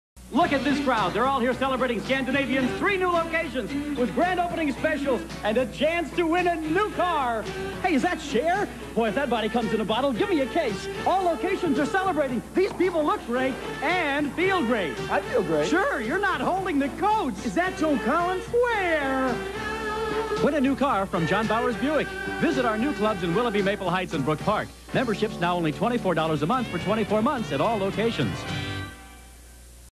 John Lanigan for Scandinavian Health Spas - 1987!
From early 1987, here is a TV commercial for Scandinavian Health Spas with Cleveland icon (now retired) John Lanigan!